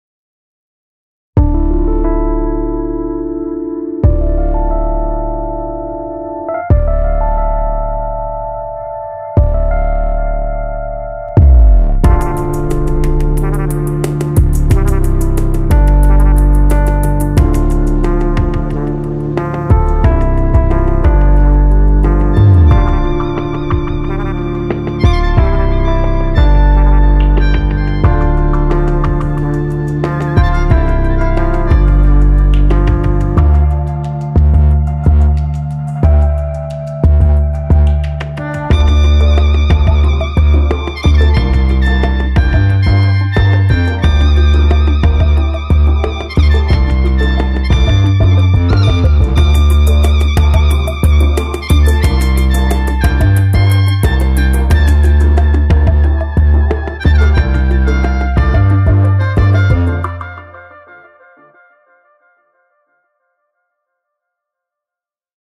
Add phase-perfect sub bass and kicks with Subcraft. Tight, powerful low-end for any genre. ✨
Releasing tomorrow! Big thanks to @ThisisAudilia for the amazing video